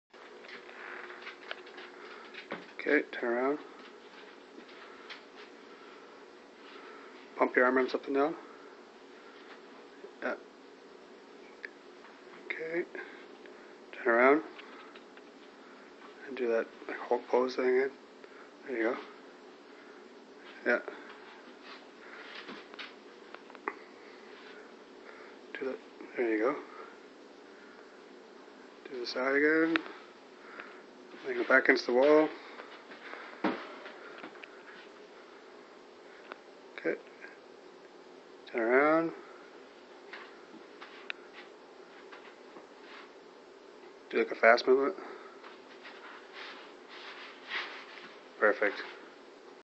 carnage costume wip -movement test
completed muscle suit for carnage costume build.